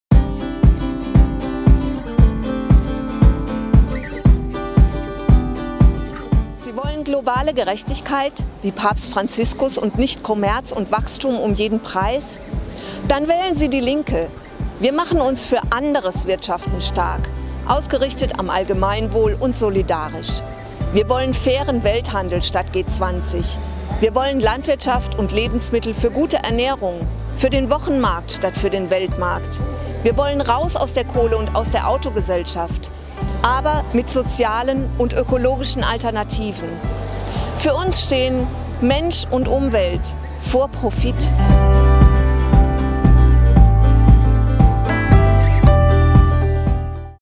Mensch und Umwelt vor Profit!
Sabine Leidig, Platz eins unserer Landesliste zur Bundestagswahl, stellt eines ihrer wichtigsten Anliegen vor.